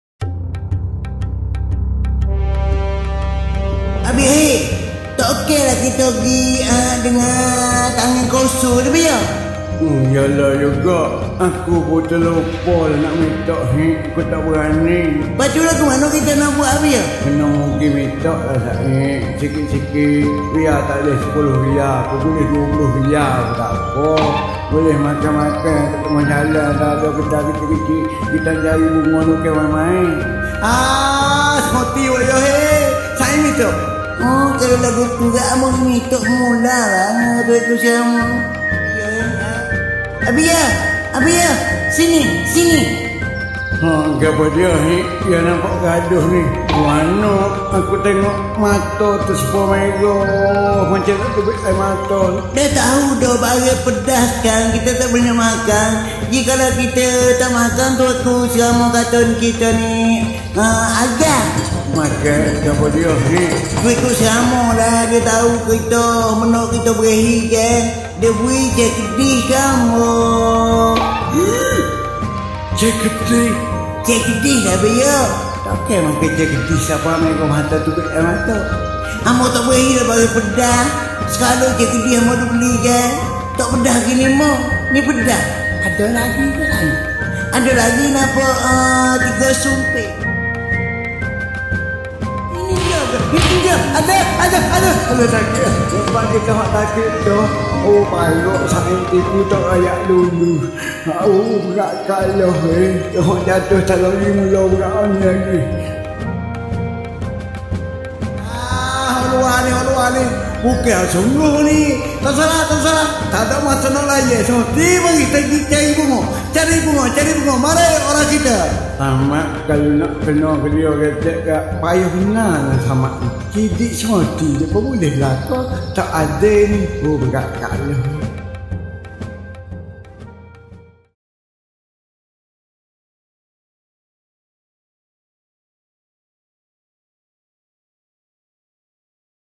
Adakah Wokyoh Samad Said sesuai dengan 3D?
Perjalanan untuk menyediakan animasi 3D memang perlu modal yang besar..Adakah Artmedia mampu?
Bagi abroy, jika ada rezeki kita on...dan animasi 2D masih diteruskan
seperti biasa..
Moga capai target insyaallah.. teruskan menyokong ya!..